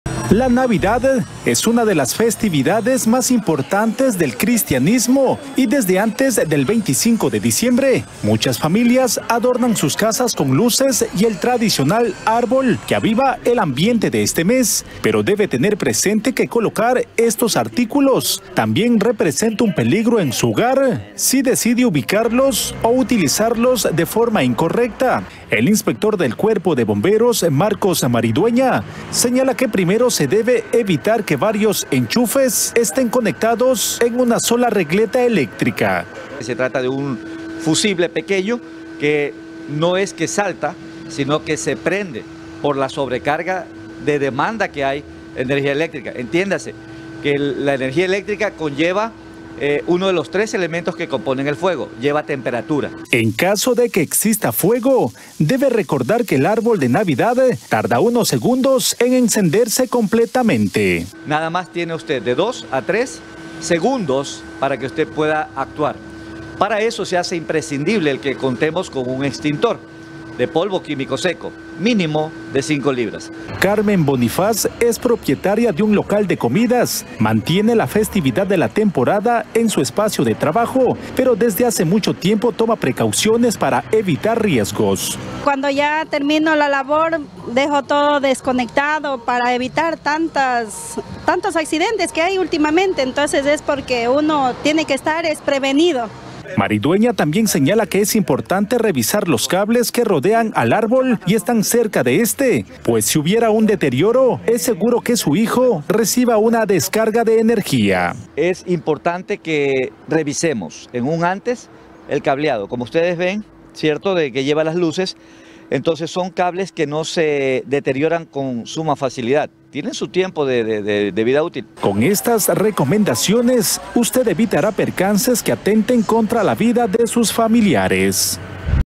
Cuerpo de Bomberos brinda consejos para evitar incendios en temporada navideña
El inspector del Cuerpo de Bomberos, Marcos Maridueña, señala que primero se debe evitar que varios enchufes estén conectados en una sola regleta eléctrica.

En caso de que el fuego, debe recordar que el árbol de Navidad tarda unos segundos en encenderse completamente.

Carmen Bonifaz, es propietaria de un local de comidas, mantiene la festividad de la temporada en su espacio de trabajo, pero desde hace mucho tiempo toma precauciones para evitar riesgos.